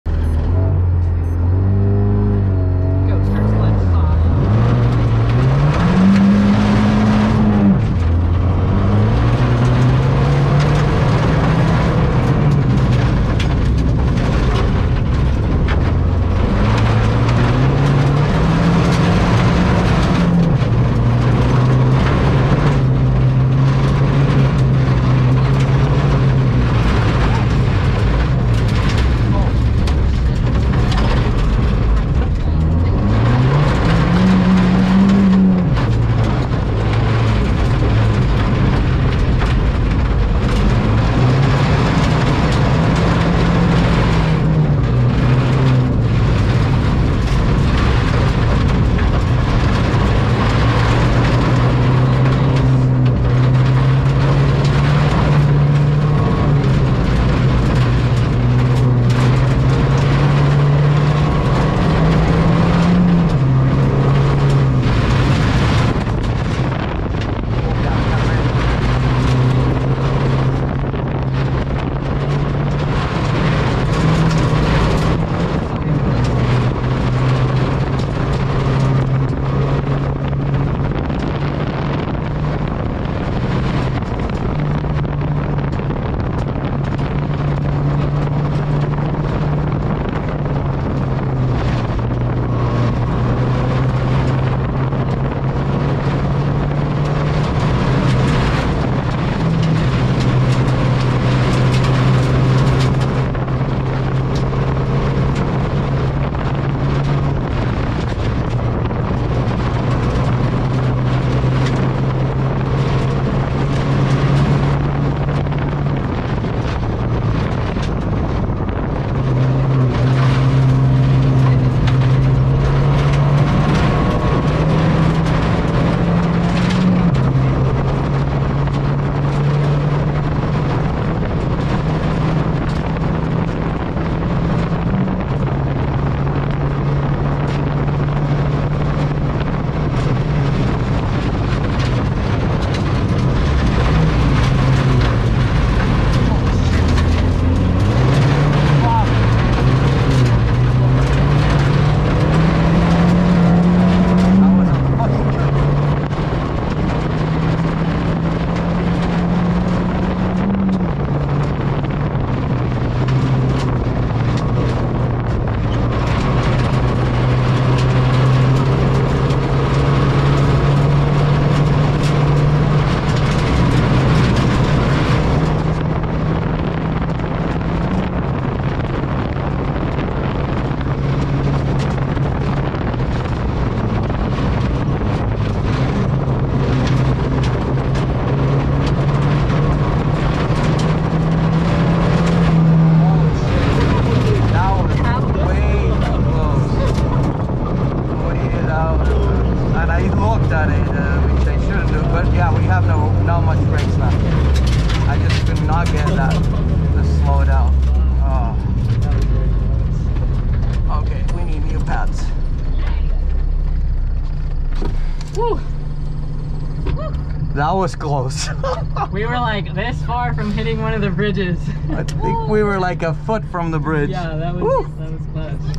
Ford Fiesta ST rally car onboard - bridge near miss - 2022 SOFR - SS5 - Zorn's Hollow
Roberto Yglesias and KJ Miller from Pura Vida Rally tackle stage 5 of Southern Ohio Forest Rally 2022 in their Ford Fiesta ST rally car, winning their class on this stage by over 8 seconds and very narrowly missing a bridge after brakes failed at the end of a long straight.